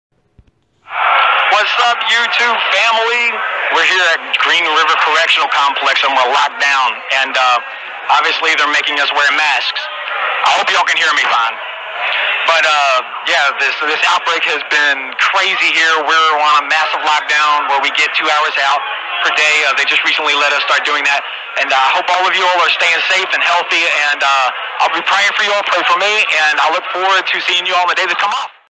KY Prison Green River COVID-19, Coronavirus, Wuhan Virus - Inmate Epperson
KY Prison Green River COVID-19, Coronavirus, Wuhan Virus

Currently, Matt is at Green River Corr. Complex in KY.

Matthew Epperson #284812
Green River C.C.